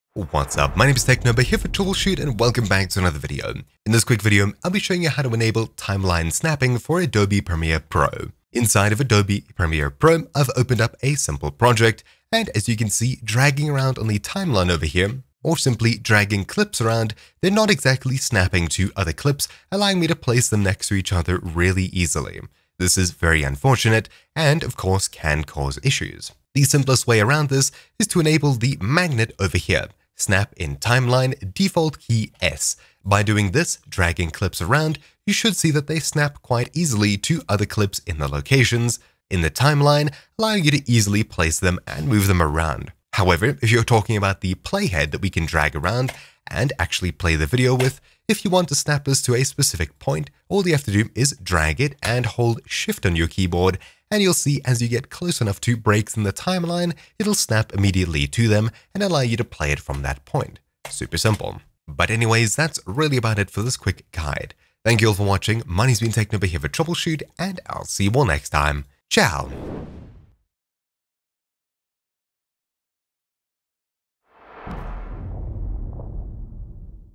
Enable timeline snapping in Premiere Pro | Fix Guide | Updated
Want clips to snap to other clips in the Premiere Pro Timeline, but they won't? Want the playhead to snap to the beginning and end of clips in the Premiere Pro timeline? This video shows you everything you need to know about fixing it, or activating the "magnet" or snapping effect.

Timestamps:
0:00 - Explanations
0:26 - Fix snapping not working in Premiere Pro
0:42 - Fix Playhead not snapping in Premiere Pro